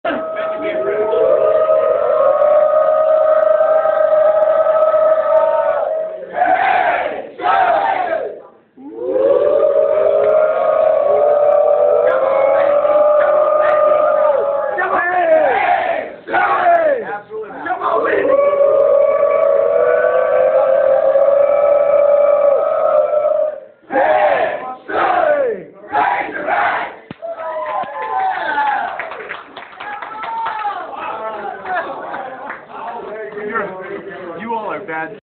Call those hogs - Commander's Palace Sugar Bowl 2011.
Call those hogs!   Dining room at Commander's Palace in New Orleans calling the hogs for the Sugar Bowl 2011 against Ohio State!